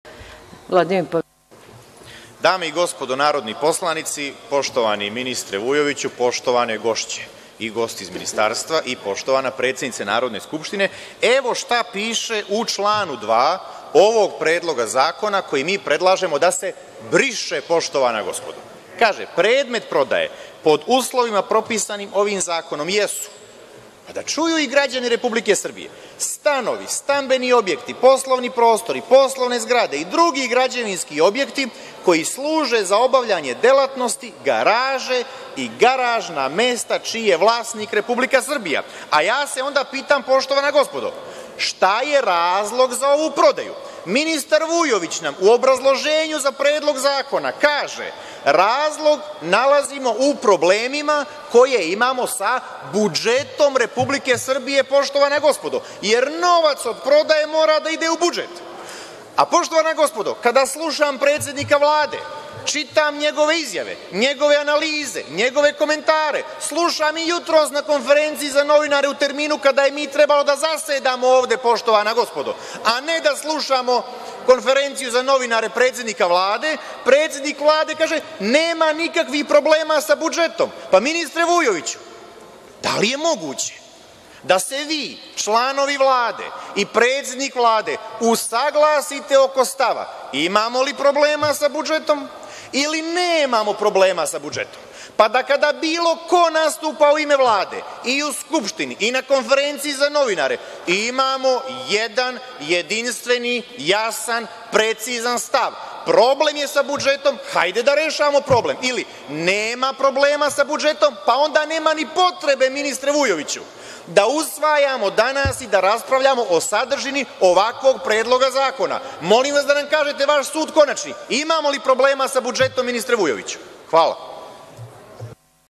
29.07.2015. – Pavićević: Usaglasite se, imamo li problem sa budžetom ili nemamo!
Vladimir Pavićević, narodni poslanik i zamenik predsednika Nove stranke

Sednica: Šesnaesto vanredno zasedanje Narodne skupštine Republike Srbije u 2015. godini